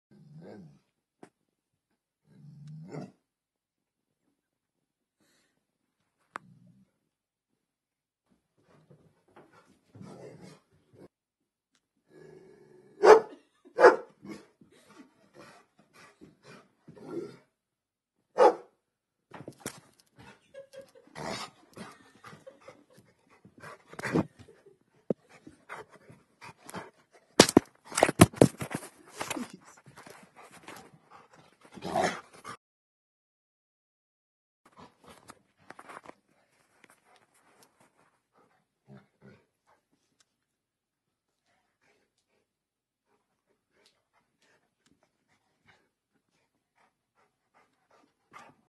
Vicious Bullmastiff Attack
Charlie just goes nuts and attacks!!